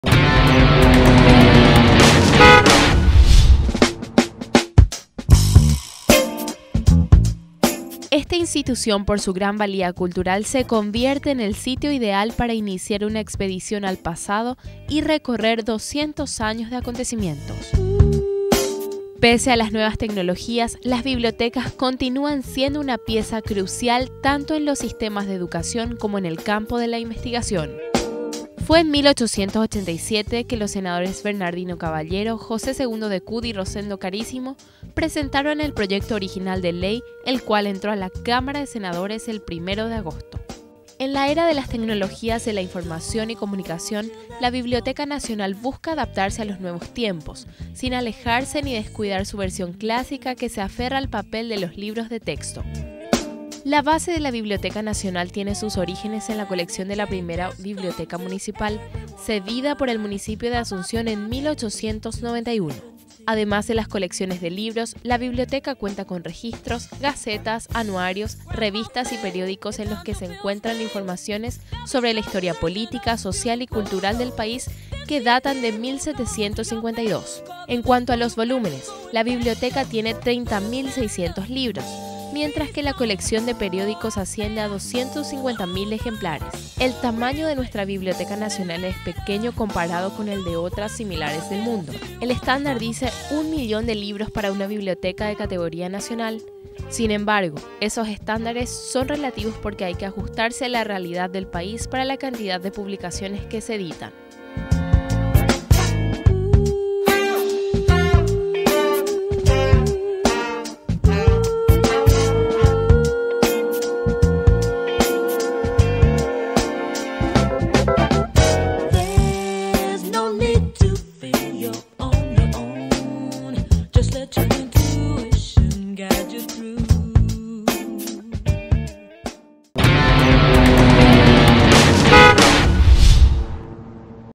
El Tour Sabores visita la Biblioteca Nacional. 31-07-2014
En esta semana visitamos la Biblioteca Nacional.